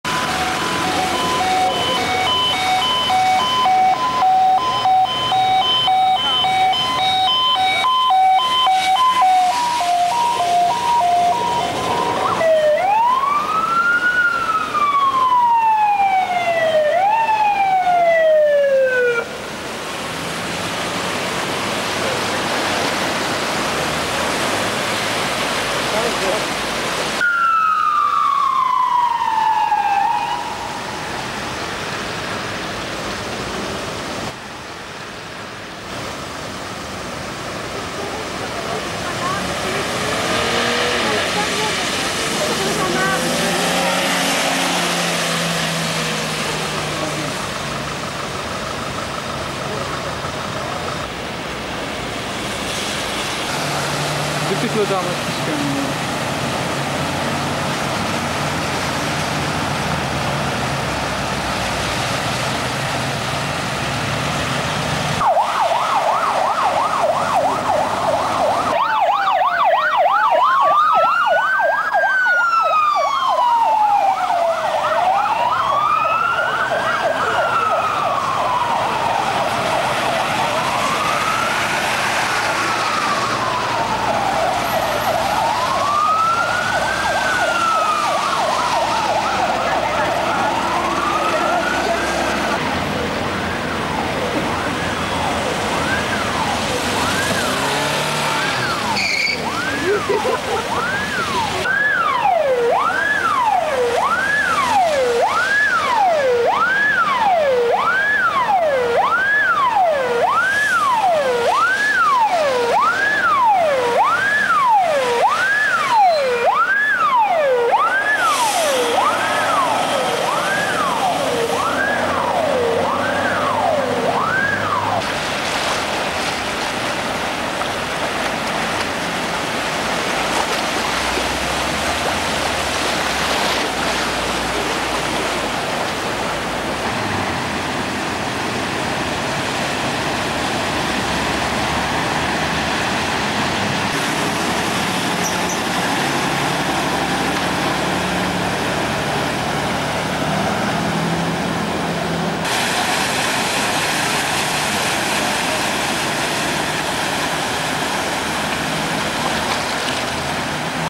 8.8.2002 - Czech emergency vehicles drive through flood water
Various emergency vehicles used in 2002 during flood, almost all of them are now retired.